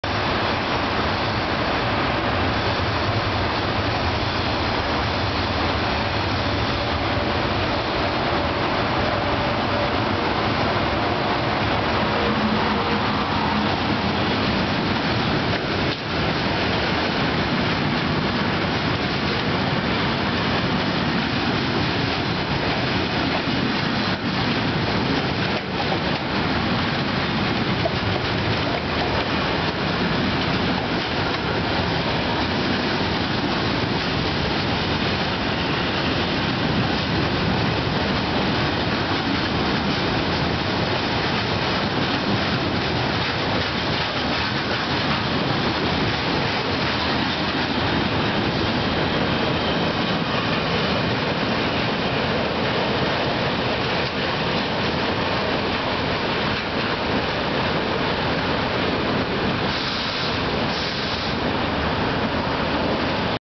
MRS Fiorentina Piombino - Roma Smistamento (Rotaie)
in coda anche due carri con garitta utilizzata in tempi remoti, anni luce, dai frenatori... transito a Tiburtina